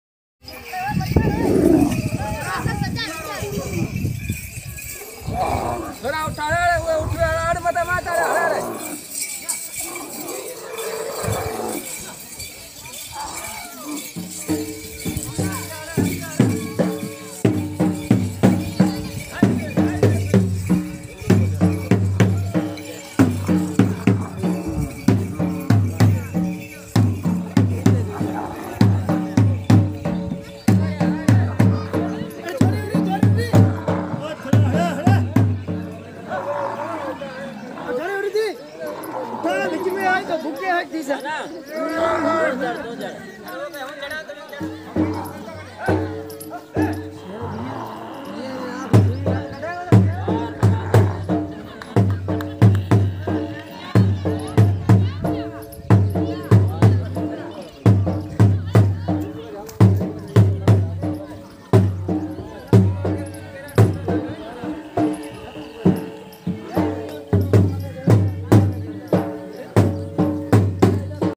Desert tharparker Camels festival || Camels Dancing || Camel beautiful Confluence
Camel baby successfully dance
Dancing Camels festival Desert tharparker pakistan lifestyle
Camels so beautiful dancing